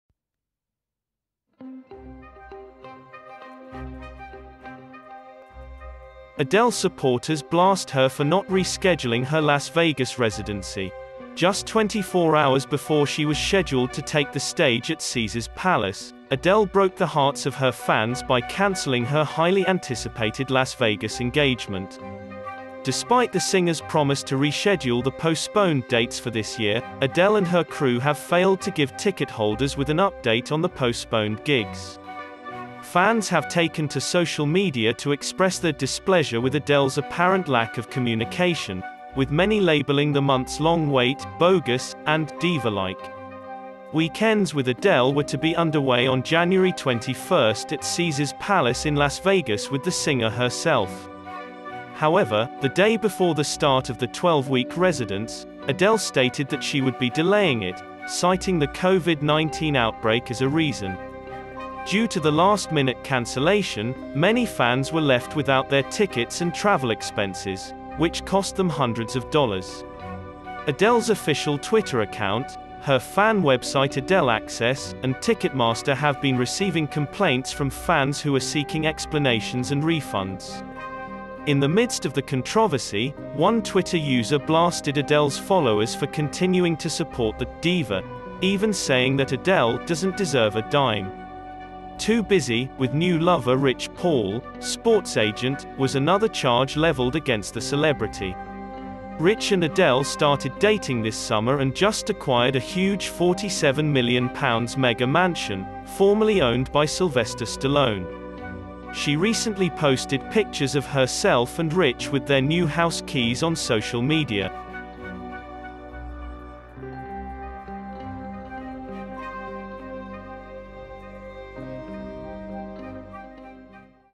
Adele supporters blast her for not rescheduling her Las Vegas residency | US News | NewsRme
Just 24 hours before she was scheduled to take the stage at Caesar's Palace, Adele broke the hearts of her fans by canceling her highly anticipated Las Vegas engagement.

Despite the singer's promise to reschedule the postponed dates for this year, Adele and her crew have failed to give ticketholders with an update on the postponed gigs.
Fans have taken to social media to express their displeasure with Adele's apparent lack of communication, with many labeling the months-long wait 'bogus' and 'diva-like.'
Weekends with Adele were to be underway on January 21 at Caesar's Palace in Las Vegas with the singer herself.